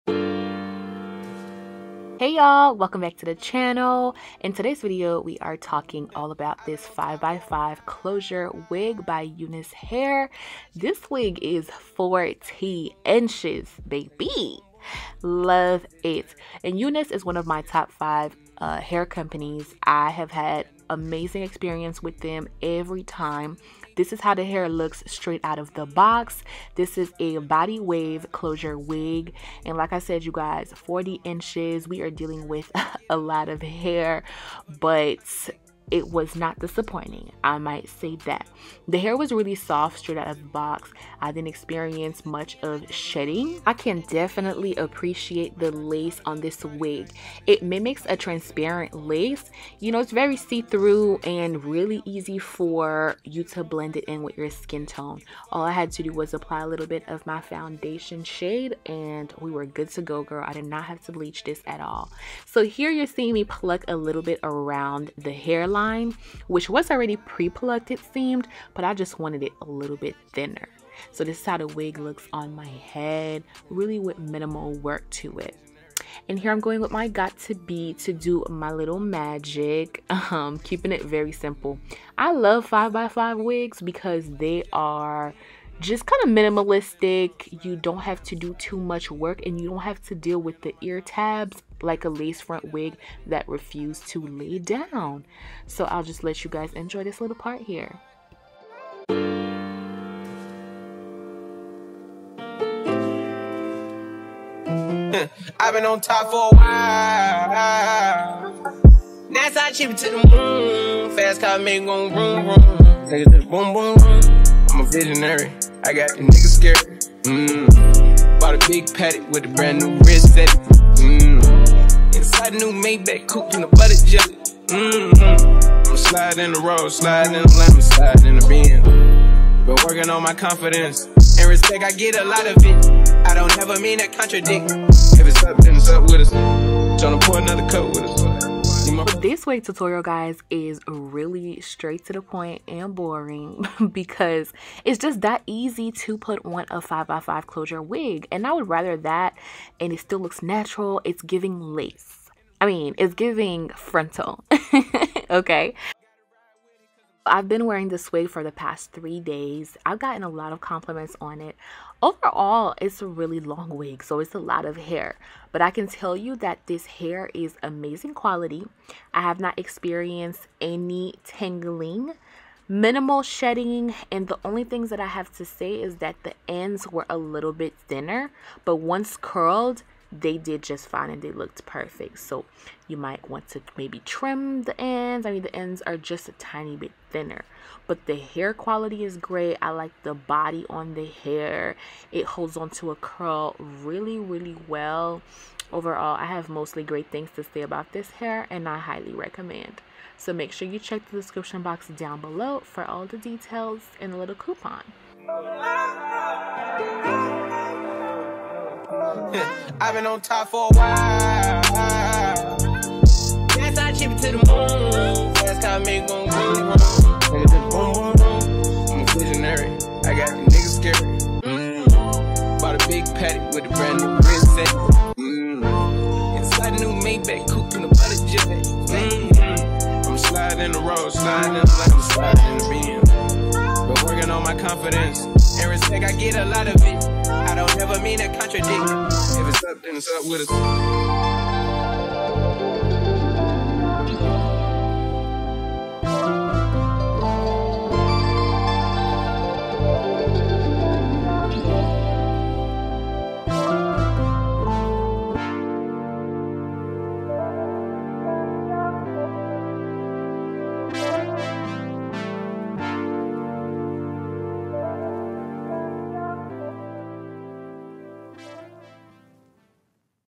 LACE WHERE ? 40 INCHES VOLUMINOUS CURLS 5x5 HD CLOSURE WOG INSTALL FT UNICE HAIR
Direct link to the Body Wave HD Lace wig in the video
Hair detail: 40 inch 180% density 5*5 HD lace Body Wave wig

Quadpay & Sezzle Now. PAY 4 Equal payment Within 6 weeks!